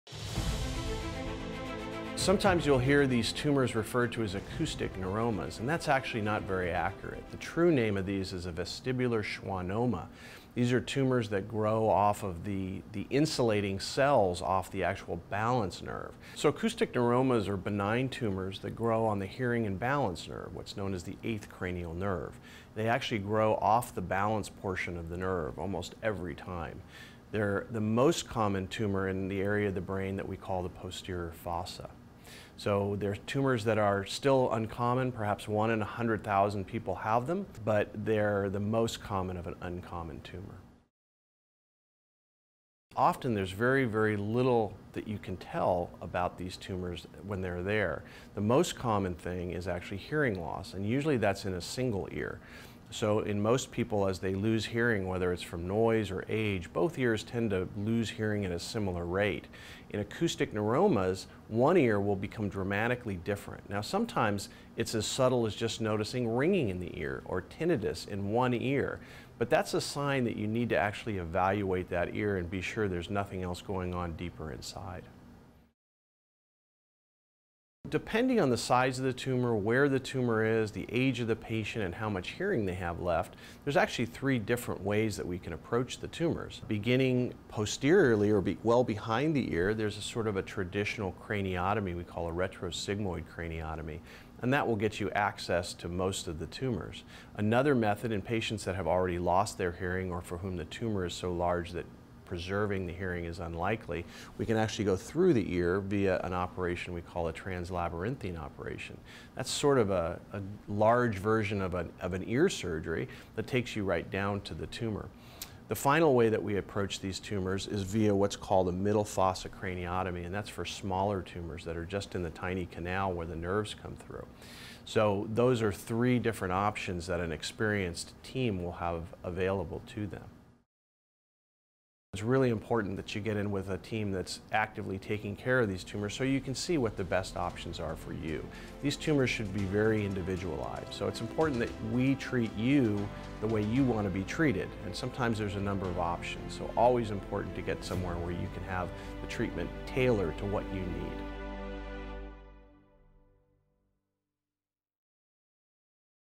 Dr. Peter Weisskopf / Dr. Bernard Bendok: Acoustic neuroma
Acoustic neuroma, also known as vestibular schwannoma, is a noncancerous and usually slow-growing tumor that develops on the main (vestibular) nerve leading from your inner ear to your brain. Branches of this nerve directly influence your balance and hearing, and pressure from an acoustic neuroma can cause hearing loss, ringing in your ear and unsteadiness. Acoustic neuroma usually arises from the Schwann cells covering this nerve and grows slowly or not at all. Rarely, it may grow rapidly and become large enough to press against the brain and interfere with vital functions. Peter Weisskopf, M.D., Mayo Clinic neurosurgeon, explains what acoustic neuroma is, symptoms and treatments.